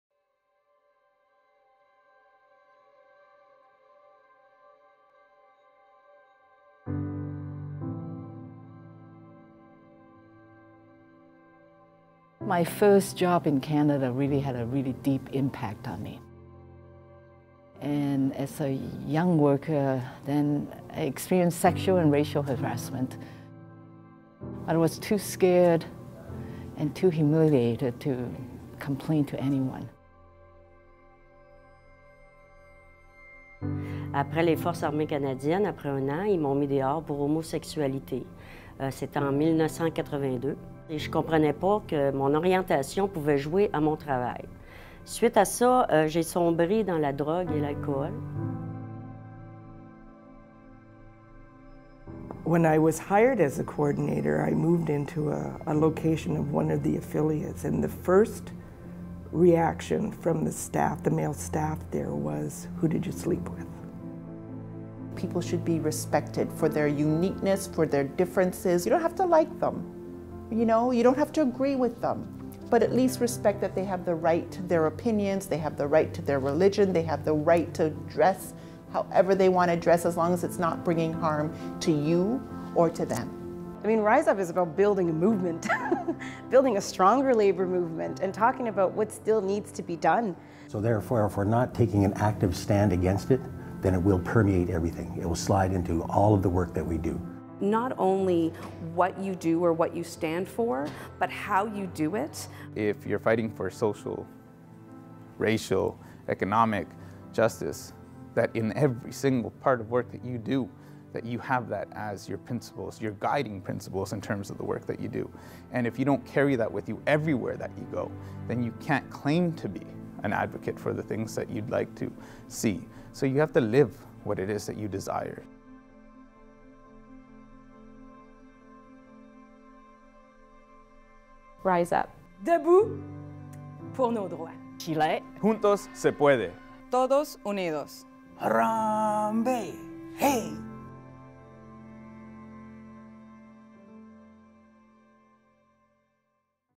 Rise Up! 2016 - Live the change
We are making change and building a stronger labour movement that recognizes the struggles of all people facing inequality in the workplace and society. The Rise Up conference is an opportunity for union activists to come together and explore equality and fairness for all.

We can empower each other by acknowledging our struggles, celebrating our gains and sharing our stories. As human rights activists, we celebrate diversity and use our collective power to create real change for all Canadians.

Share your story. Be the change. Live the change.